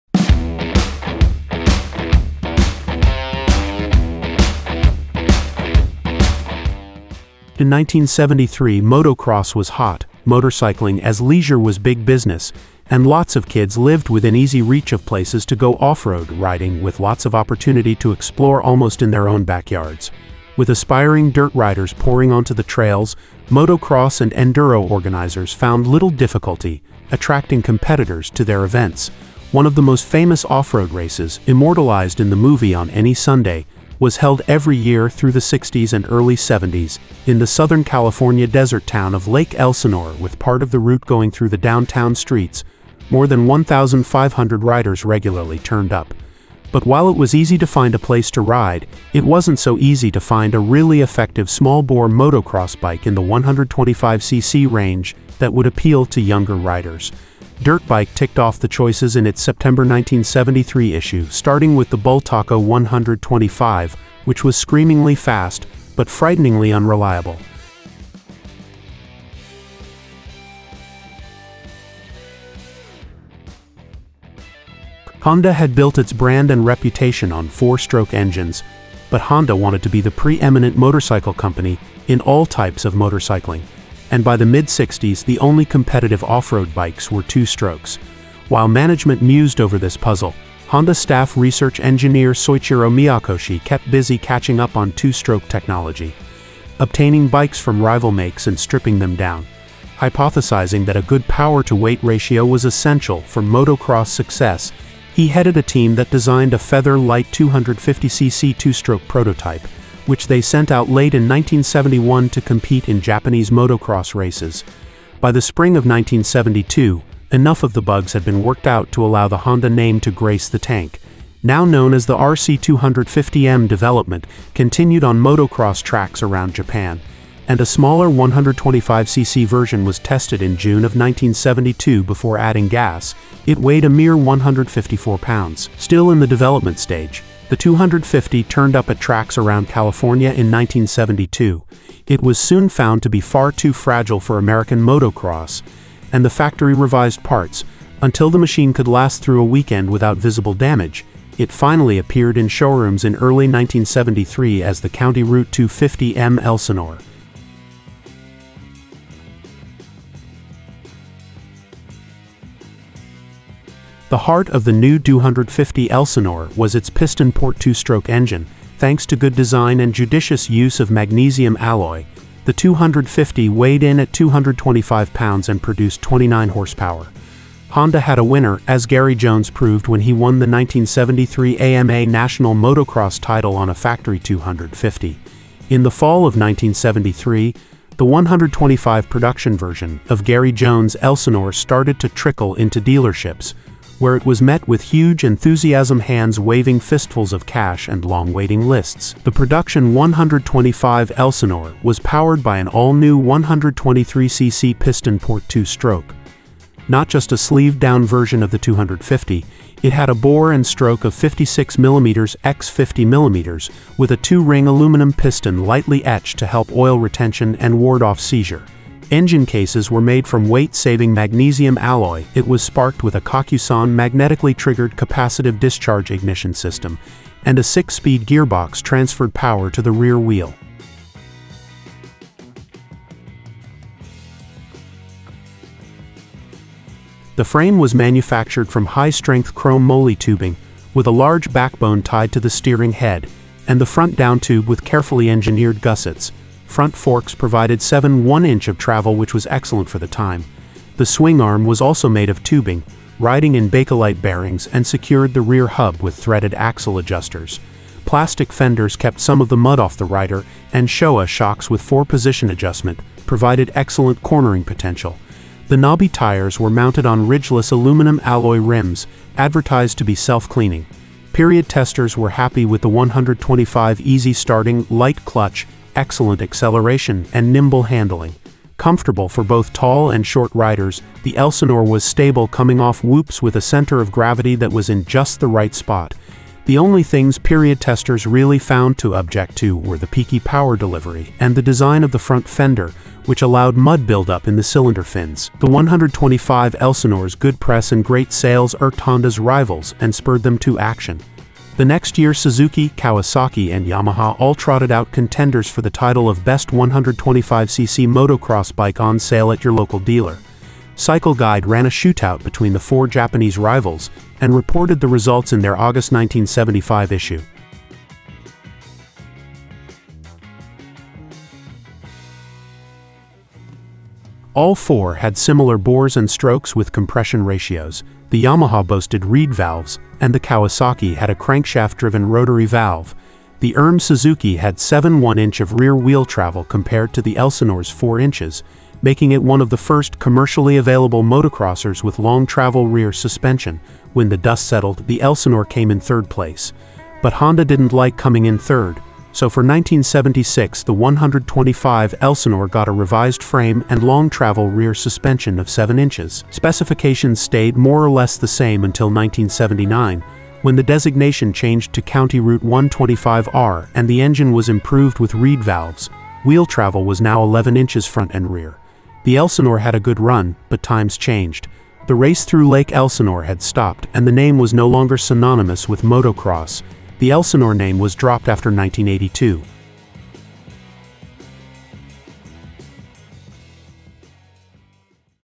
Honda CR125M Elsinore Honda Staff Engineer Soichiro Miyakoshi Kept Busy Catching Up On 2 Stroke
In 1973 motocross was hot, Motorcycling as leisure was big business and lots of kids lived within easy reach of places to go offroad riding with lots of opportunity to explore almost in their own backyards. With aspiring dirt riders pouring onto the trails, motocross and enduro organizers found little difficulty attracting competitors to their events. One of the most famous offroad races, immortalized in the movie On Any Sunday was held every year through the Sixties and early Seventies in the Southern California desert town of Lake Elsinore with part of the route going through the downtown streets, More than 1,500 riders regularly turned up. But while it was easy to find a place to ride, it wasn’t so easy to find a really effective small bore motocross bike in the 125cc range that would appeal to younger riders. Dirt Bike ticked off the choices in its September 1973 issue starting with the Bultaco 125 which was screamingly fast, but frighteningly unreliable. Honda had built its brand and reputation on 4 stroke engines, But Honda wanted to be the pre eminent motorcycle company in all types of motorcycling, and by the mid Sixties the only competitive offroad bikes were 2 strokes. While management mused over this puzzle, Honda staff research engineer Soichiro Miyakoshi kept busy catching up on 2 stroke technology, obtaining bikes from rival makes and stripping them down. Hypothesizing that a good power to weight ratio was essential for motocross success, he headed a team that designed a feather light 250cc 2 stroke prototype, which they sent out late in 1971 to compete in Japanese motocross races. By the spring of 1972 enough of the bugs had been worked out to allow the Honda name to grace the tank. Now known as the RC 250 M development continued on motocross tracks around Japan, and a smaller 125cc version was tested in June of 1972 before adding gas, it weighed a mere 154 pounds. Still in the development stage, the 250 turned up at tracks around California in 1972. It was soon found to be far too fragile for American motocross and the factory revised parts until the machine could last through a weekend without visible damage, It finally appeared in showrooms in early 1973 as the CR 250 M Elsinore.